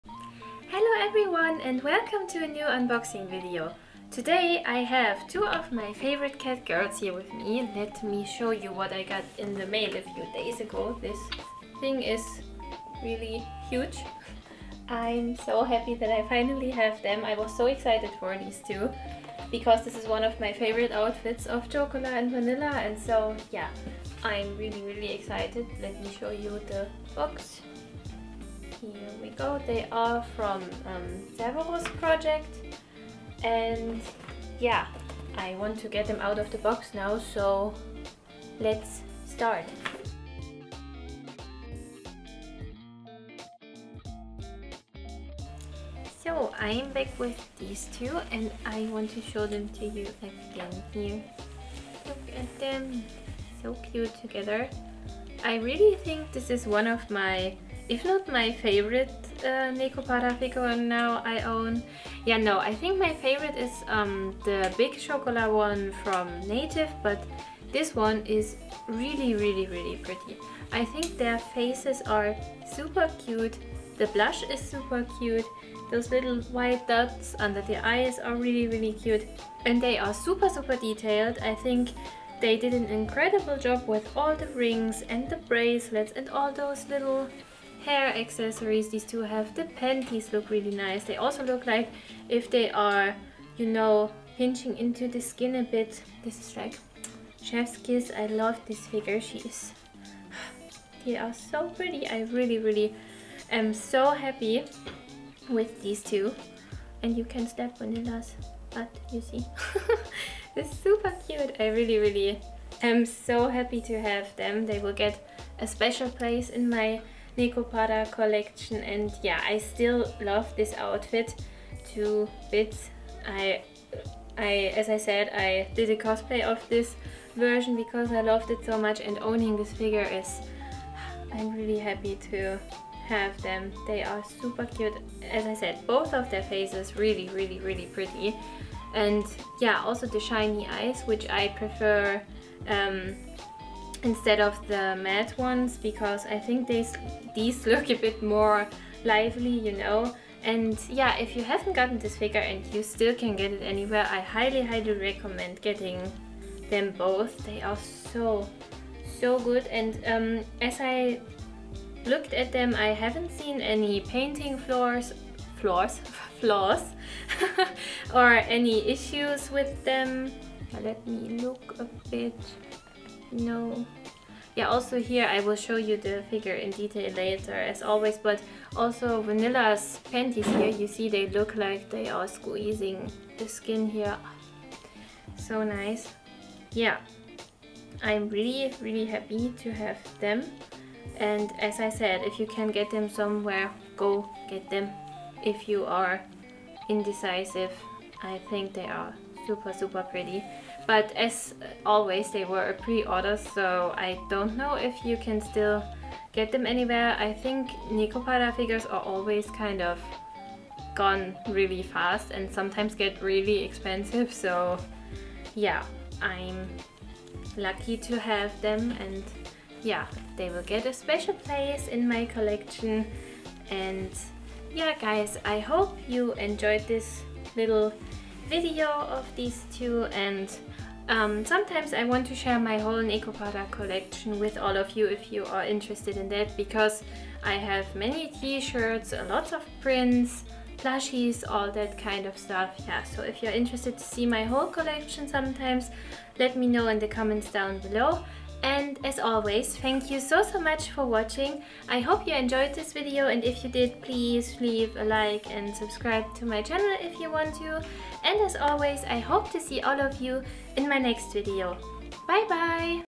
Is this the perfect Nekopara figure?
Grail-tier figure of the cutest cat girls, Chocola and Vanilla! This is perfection with cat ears and very sexy lingerie! Just watch the video, it's pretty short because my second camera decided to NOT record the actual unboxing part (you can hear a click in the first couple of seconds which is the camera shutting off xD). Hope I can at least make it up with the quality, this might be my best looking video to date!